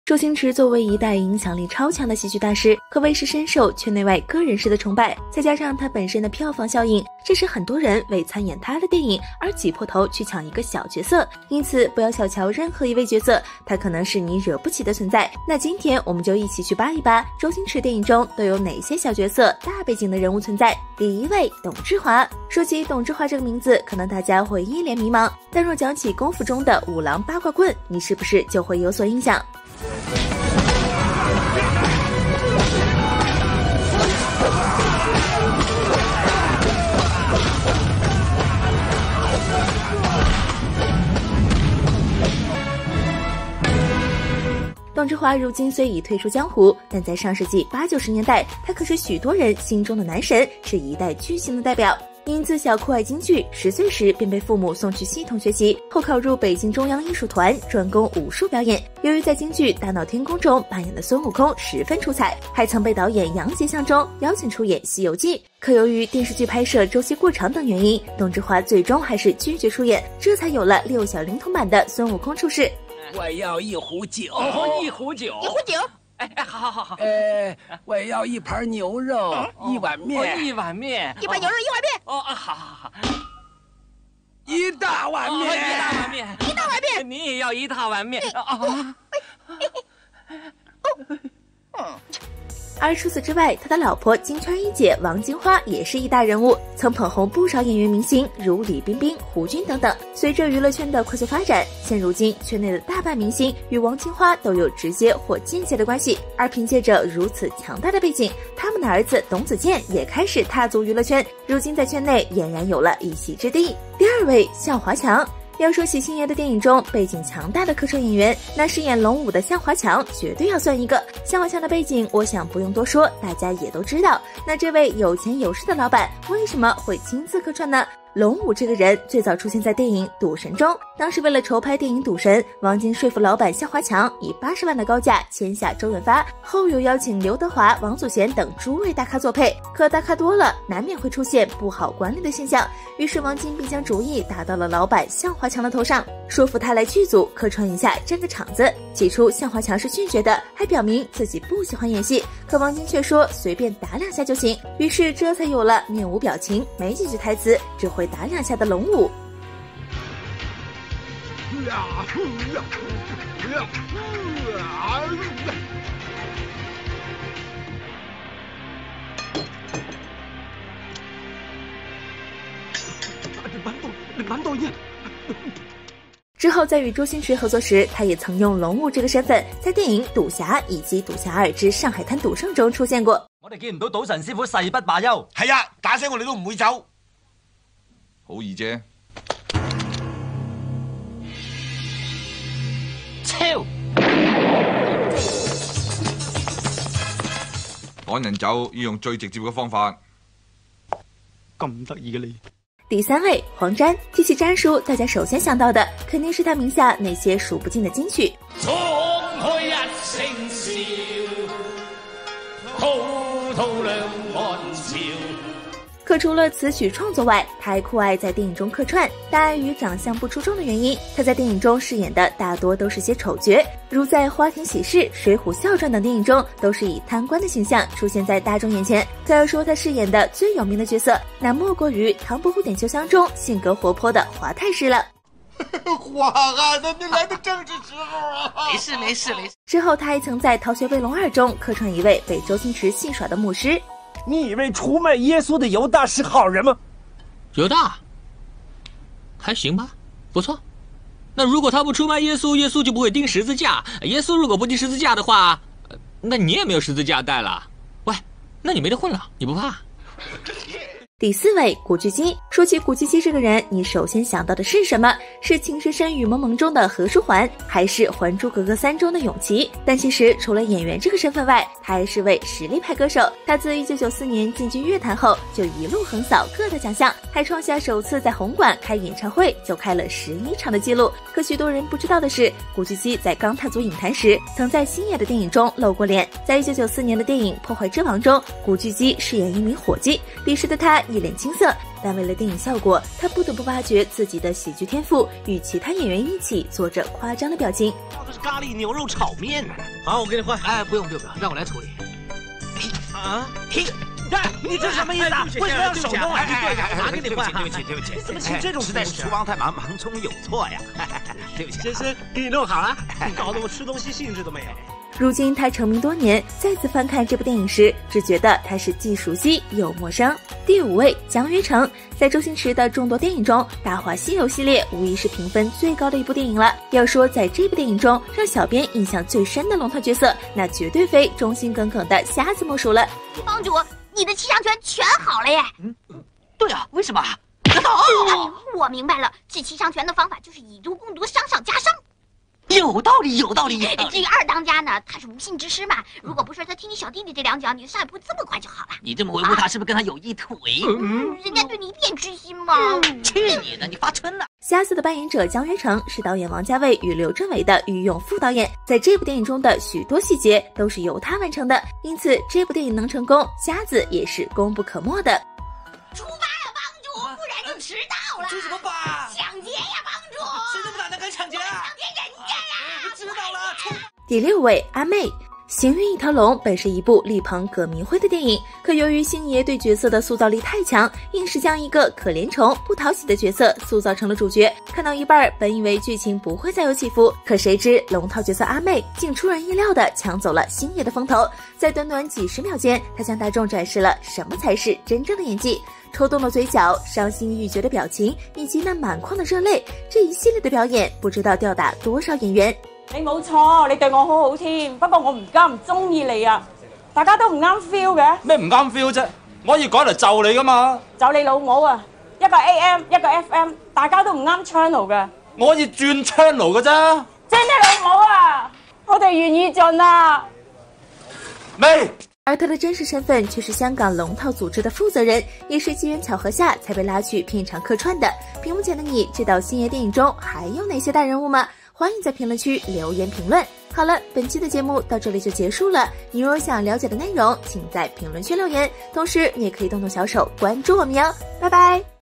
星爷电影中了不得的小人物：阿媚古巨基勉强上榜，向华强屈居第二，第一内娱无人敢惹
周星驰作为一代影响力超强的喜剧大师
可谓是深受圈内外各人士的崇拜
再加上他本身的票房效应
致使很多人为参演他的电影而挤破头去抢一个小角色
因此不要小瞧任何一位角色
他可能是你惹不起的存在
那今天我们就一起去扒一扒周星驰电影中
都有哪些小角色大背景的人物存在